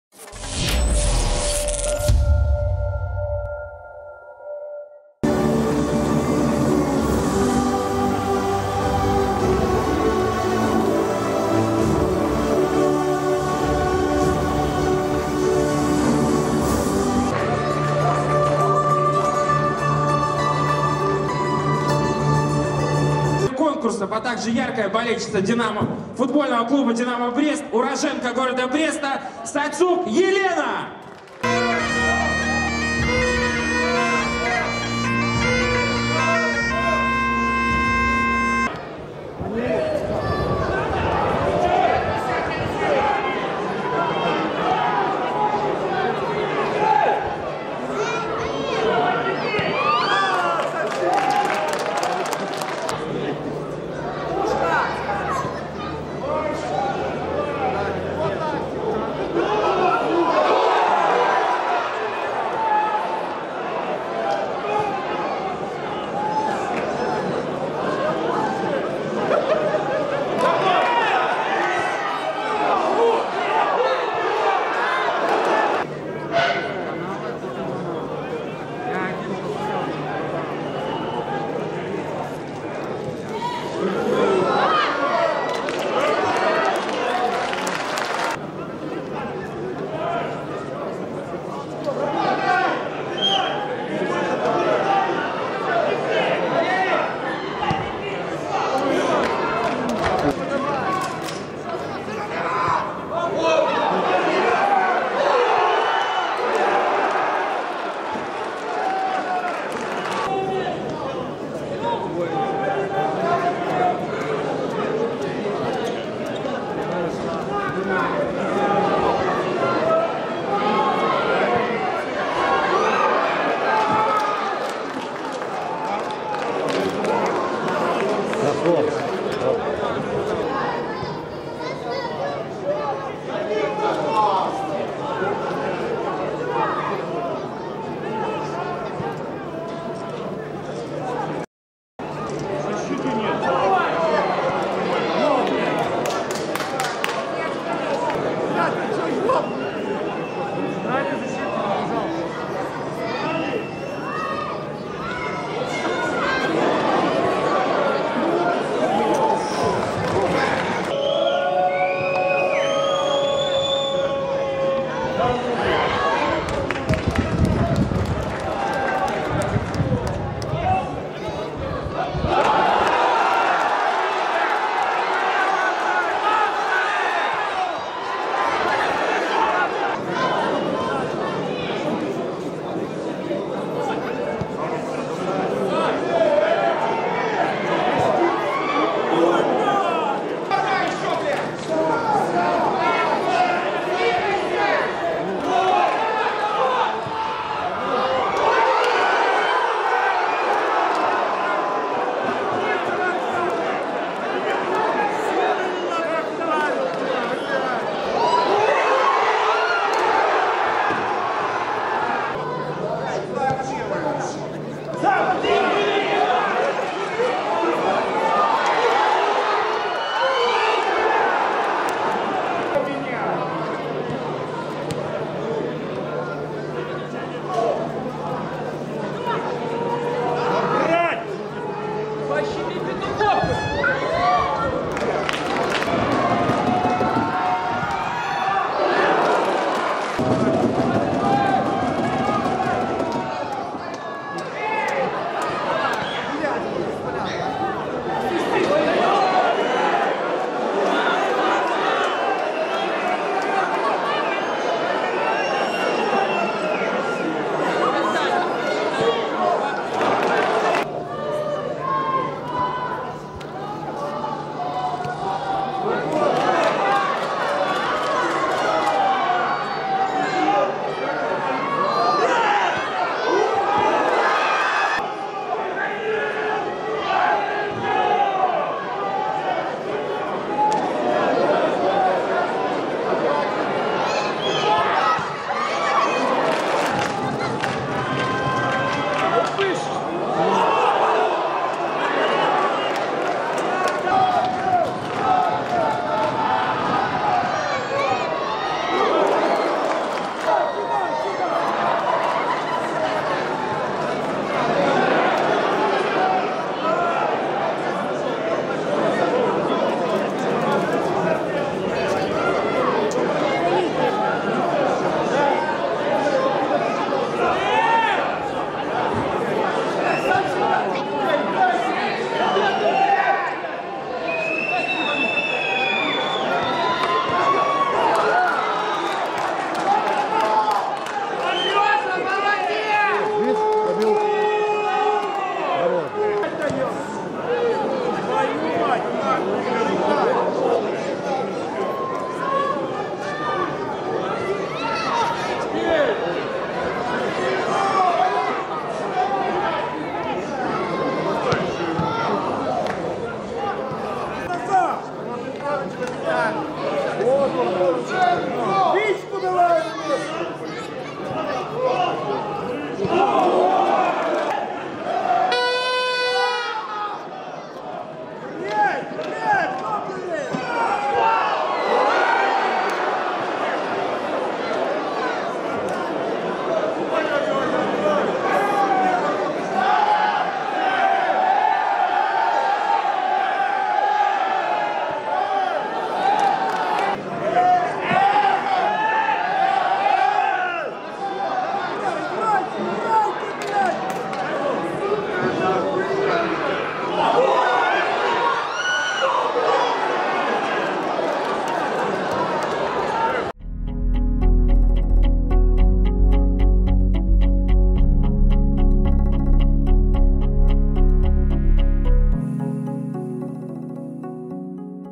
Тур 6. Динамо-Брест - Шахтер - 0:2 (25.04.2020)
Динамо-Брест: Павлюченко, Юзепчук, Веретило (к), Павловец, Витус (Быков, 70), Кисляк, Нойок, Савицкий (Шевченко, 85), Милевский, Гордейчук (Кривец, 74), Лаптев.

Шахтер: Гутор (к), Вукманич, Хадаркевич, Сачивко, Антич, Сзоке, Селява, Иванович, Кендыш (Сотников, 85), Баланович (Подстрелов, 90), Лисакович (Бодул, 77).

Голы: 0:1 — Витус (26, автогол, пас — Антич). 0:2 — Лисакович (67, пас — Иванович).

Предупреждения: Юзепчук (21), Нойок (33) — Антич (33), Хадаркевич (62), Сачивко (75), Сзоке (82), Селява (86), Бодул (92+), Сотников (93+).